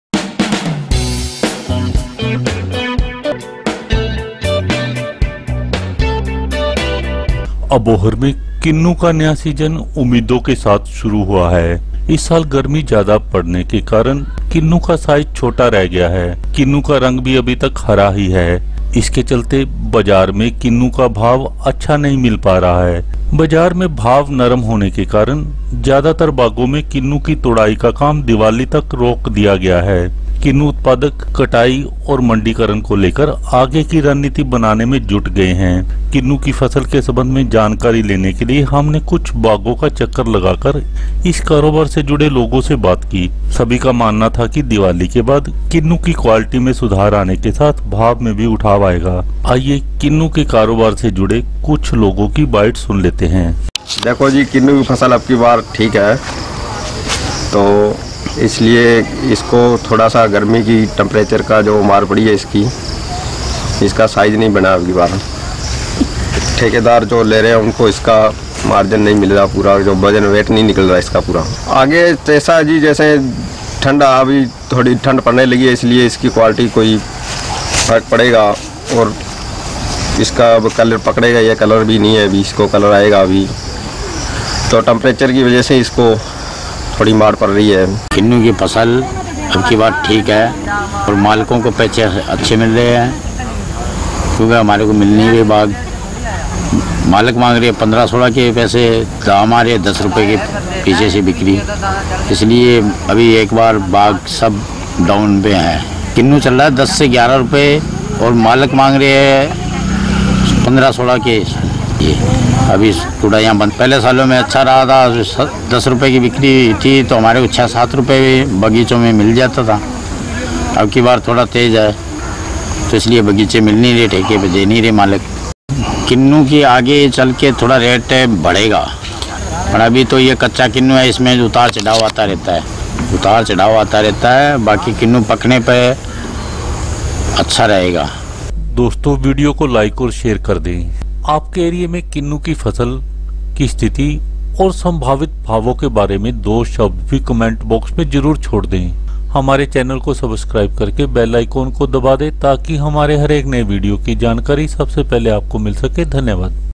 Abohar में New Kinnow Season उम्मीदों के साथ शुरू हुआ
This video is about new kinnow season Abohar. Arrival of Kinnow has started at Kinnow Mandi Abohar. But rates of Kinnow are low due to under size and green colour. Hence Farmers and contractors has stopped harvesting of Kinnow till Deewali. Kinnow Season Abohar has started on promising note. After Deewali quality of Kinnow will improve and rates will also increase.